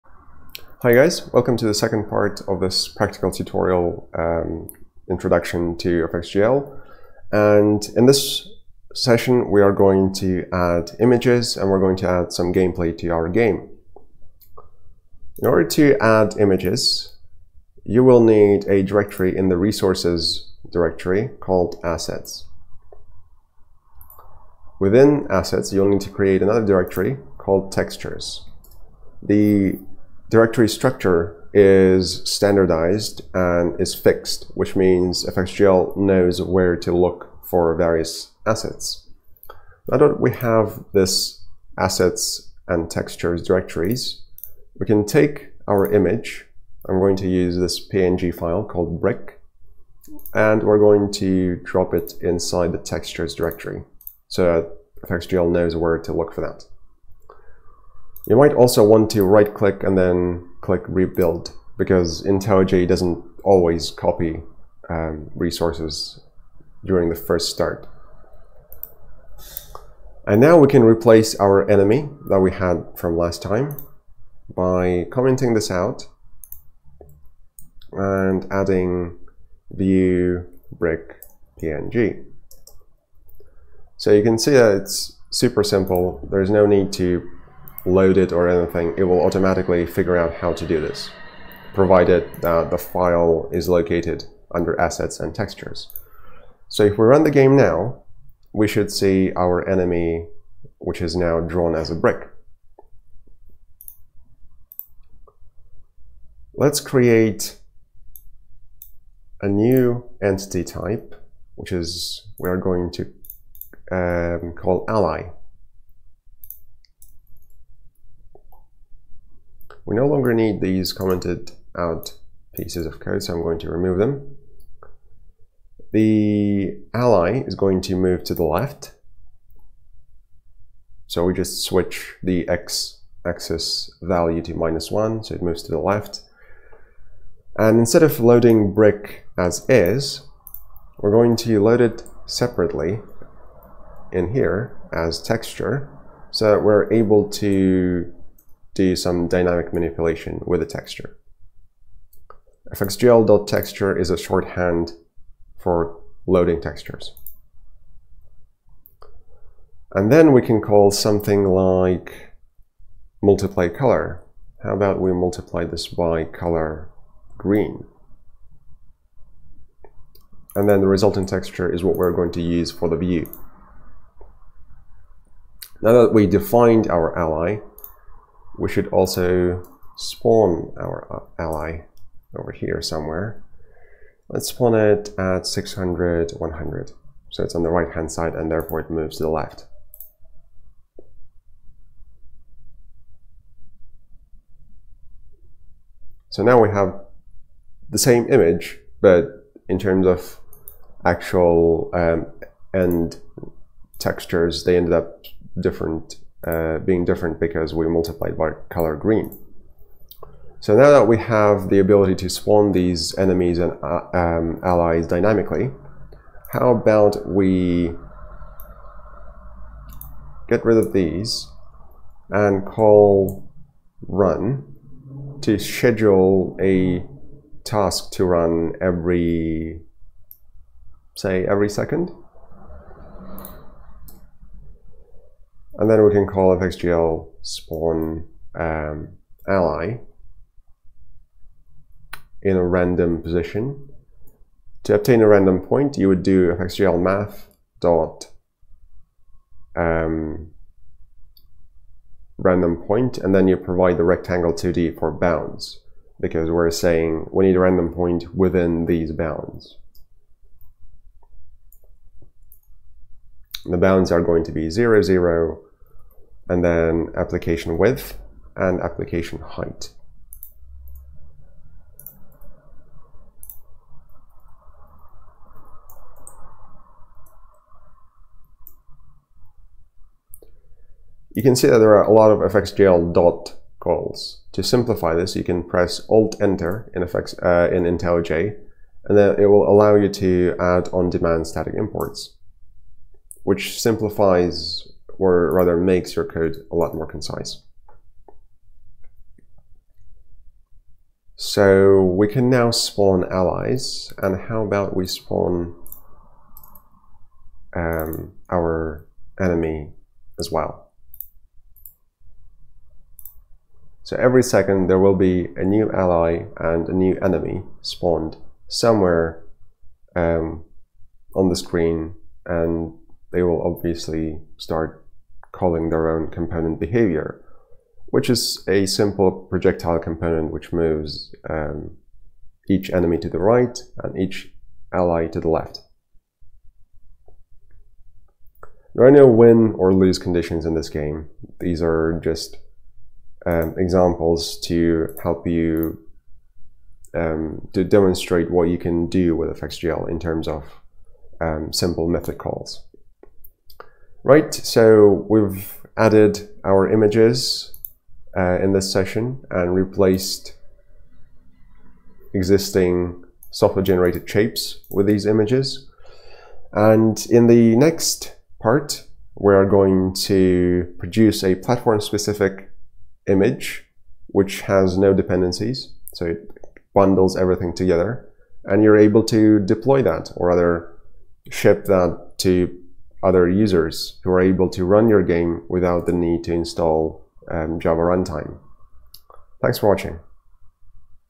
Brighton Java Workshop: Introduction to FXGL (Part 2/3)
We add textures.

or by clicking the Join button